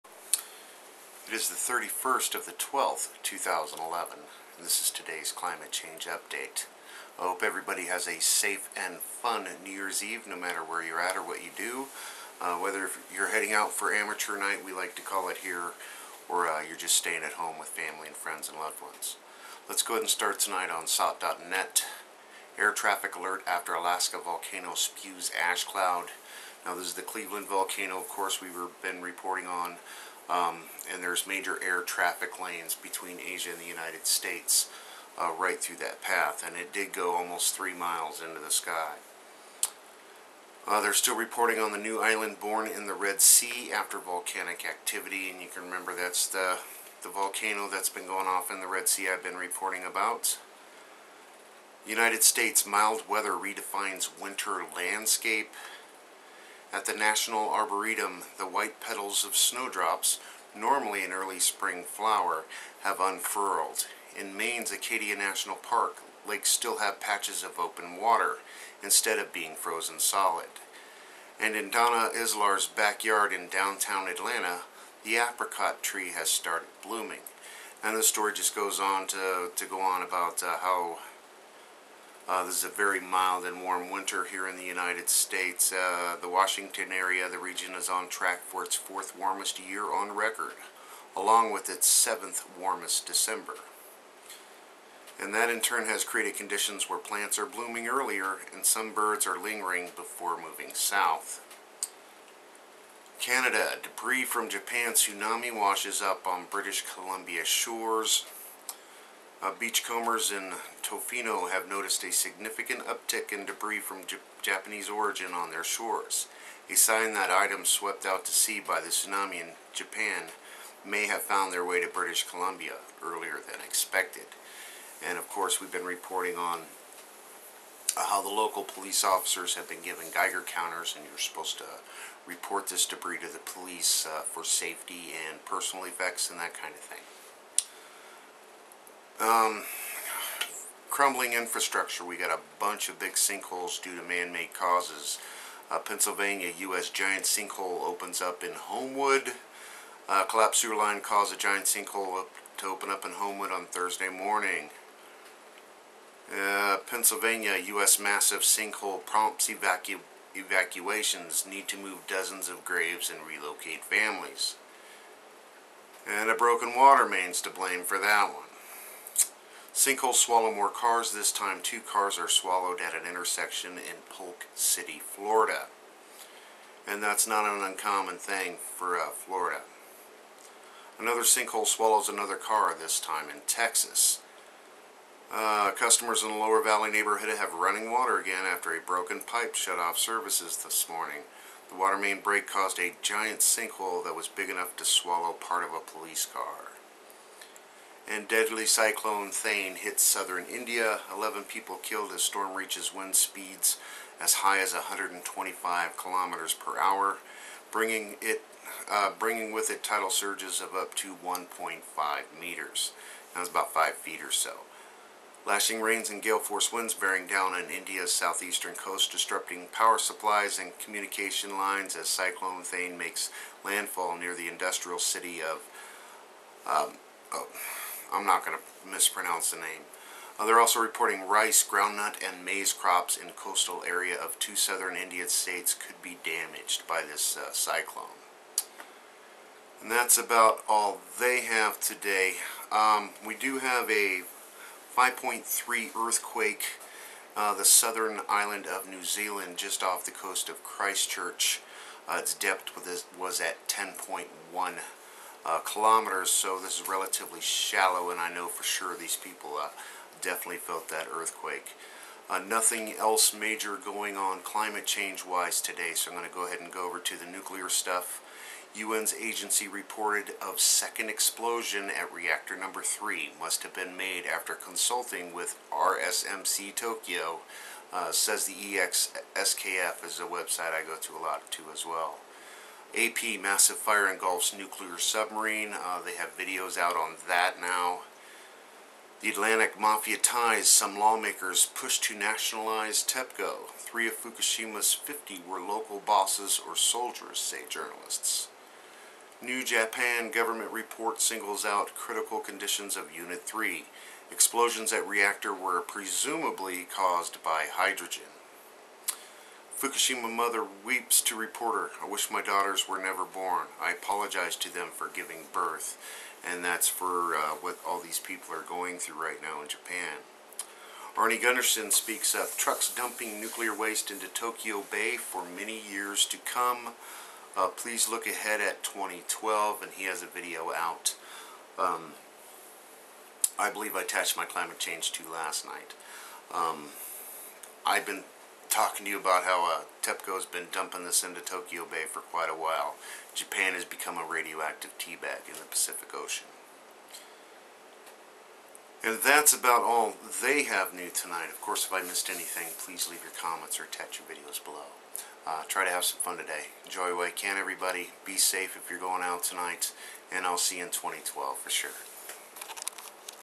Climate Change Update (31 December 2011) 5.3 Earthquake Christchurch NZ
An earthquake with a magnitude of 5.3 struck close to the New Zealand city of Christchurch on Saturday, the U.S. Geological Survey reported, but there were no immediate reports of damage or casualties.

Canada: Debris from Japan tsunami washes up on British Columbia shores

Atlantic: Mafia ties behind some lawmakers' push to nationalize Tepco — 3 of Fukushima 50 were local bosses or soldiers says journalist